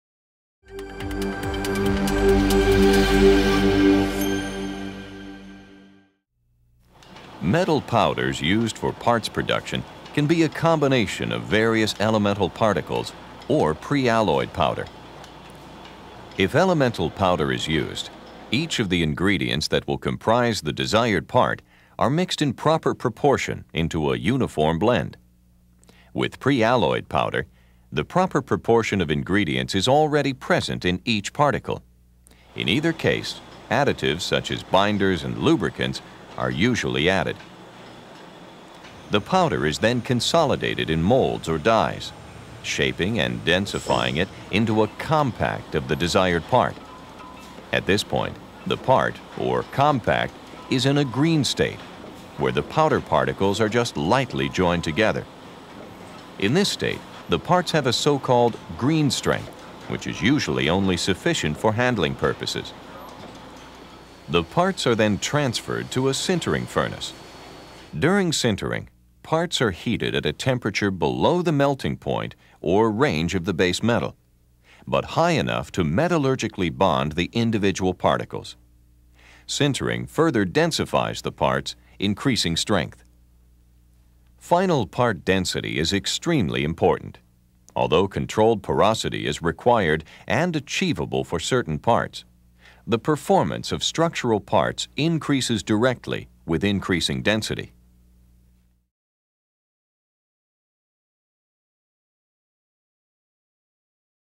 Powder Metallurgy
Powder metallurgy is a metalworking technology used for producing parts from metal powder. Advantages include part design flexibility and the ability to produce net or near-net shaped parts. Part of the Fundamental Manufacturing Processes Video Series, this program illustrates the metal powder production process and the part production processes using those powders.

The metal powder production segment looks at the various types of metal powders and the physical and mechanical methods used to produce them.

The powder metal parts production segment explores the common part production processes including mechanical pressing and sintering, metal injection molding, hot isostatic pressing, and cold isostatic pressing. Through the use of shop floor footage and animations, each of these processes is explained in detail, providing a clear understanding of the similarities and differences between each of them. Secondary operations, such as forging, sealing, plating, and heat treating are also featured.